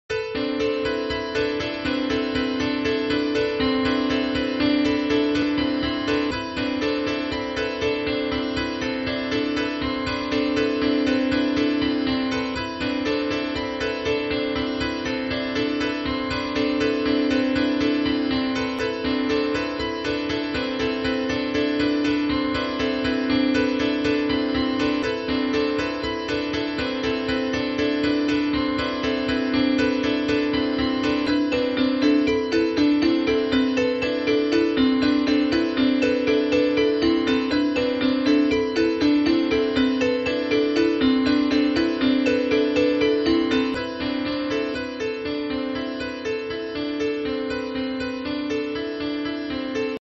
Erode sky
Eroding video of timelapse sky
modified with python , music generated by python.

original video: J. cas